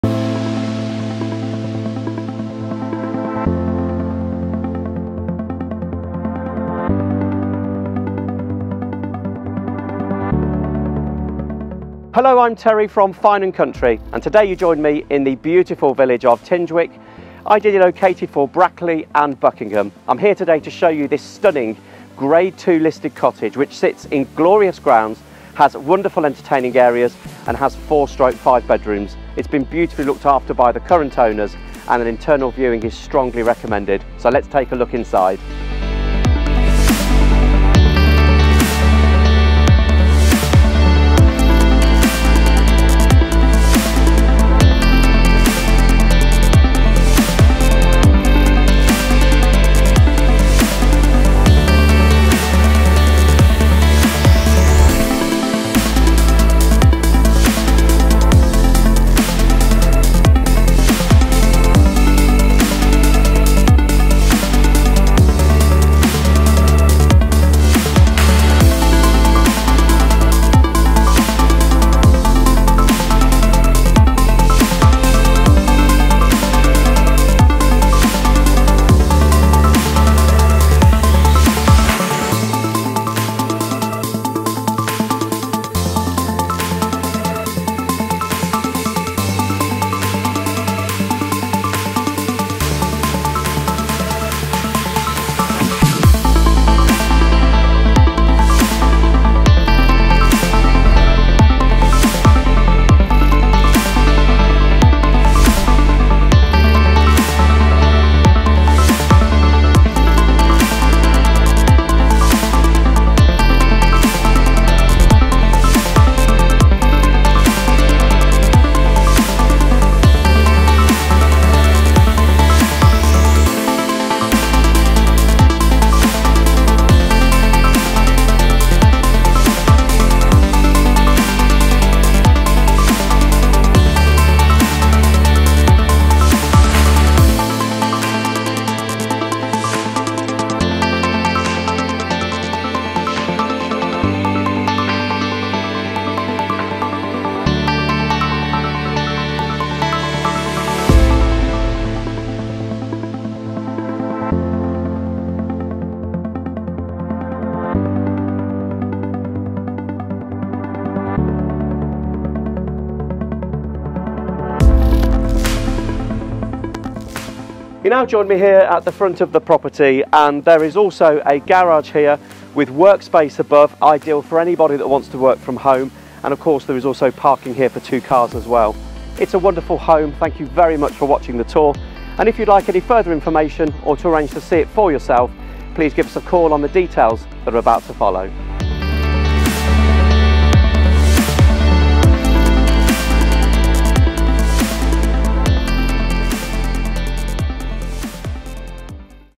‼‼ 𝗡𝗘𝗪 𝗣𝗥𝗜𝗖𝗘 𝗔𝗟𝗘𝗥𝗧 ‼‼ 📍 𝗧𝗜𝗡𝗚𝗘𝗪𝗜𝗖𝗞 | 𝗕𝗨𝗖𝗞𝗜𝗡𝗚𝗛𝗔𝗠 💰 𝗢𝗙𝗙𝗘𝗥𝗦 𝗢𝗩𝗘𝗥 | £𝟴𝟱𝟬,𝟬𝟬𝟬
🏡 𝗔 𝘀𝘁𝘂𝗻𝗻𝗶𝗻𝗴 𝗚𝗿𝗮𝗱𝗲 𝗜𝗜 𝗟𝗶𝘀𝘁𝗲𝗱 𝗰𝗼𝘁𝘁𝗮𝗴𝗲 𝗶𝗻 𝗮 𝘃𝗶𝗹𝗹𝗮𝗴𝗲 𝘀𝗲𝘁𝘁𝗶𝗻𝗴, 𝗶𝗱𝗲𝗮𝗹 𝗳𝗼𝗿 𝗕𝗿𝗮𝗰𝗸𝗹𝗲𝘆 𝗮𝗻𝗱 𝗕𝘂𝗰𝗸𝗶𝗻𝗴𝗵𝗮𝗺.🏡

Contact 𝗧𝗲𝗿𝗿𝘆 𝗥𝗼𝗯𝗶𝗻𝘀𝗼𝗻 on the details below to arrange your viewing ☟

⭐ Outstanding dining kitchen
⭐ Cloakroom and shower room
⭐ Utility room
⭐ Dining room, sitting room and sunroom
⭐ Four/five bedrooms
⭐ Two en suites and family bathroom
⭐ Garage with home office above and parking for two cars
⭐ Outstanding large rear garden with sun terrace